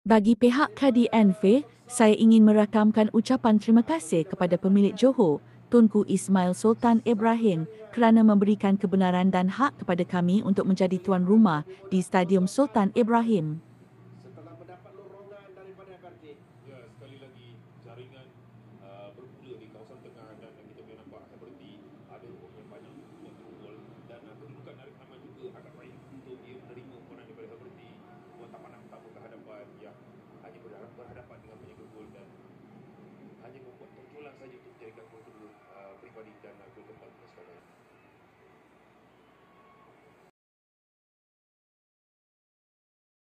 Perak fc Patut Contohi KDN fc Krn “Memohon Agar Venue Perlawanan Liga Super Dgn JDT Diubah Ke SSI.
Kelantan Darul Naim mohon SSI sebagai gelanggang tuan rumah
PASUKAN dari pantai timur, Kelantan Darul Naim FC hari ini mengesahkan telah membuat permohonan kepada pihak MFL untuk menggunakan Stadium Sultan Ibrahim sebagai gelanggang tuan rumah bagi aksi minggu ke-11 Liga Super Malaysia 2024/25.
Menerusi satu hantaran di laman sosial, Presiden KDN FC, Rozi Muhamad berkata, pihaknya telah mendapat persetujuan bersama dengan JDT berhubung perubahan venue itu susulan Stadium Sultan Muhammad IV masih dalam proses naik taraf sehingga pertengahan Oktober depan.
“Stadium Sultan Muhammad IV terpaksa ditutup buat sementara waktu bagi proses naik taraf jadi kami memohon agar venue perlawanan Liga Super dengan JDT diubah ke SSI"Bagi pihak KDN FC, saya ingin merakamkan ucapan terima kasih kepada pemilik JDT, Tunku Ismail Sultan Ibrahim kerana memberikan kebenaran dan hak kepada kami untuk menjadi tuan rumah di SSI.
“Oleh kerana lokasi kedua-dua venue berada berdekatan maka KDN FC bersetuju mengalihkan perlawanan ke SSI kerana dapat menjimatkan kos pengurusan.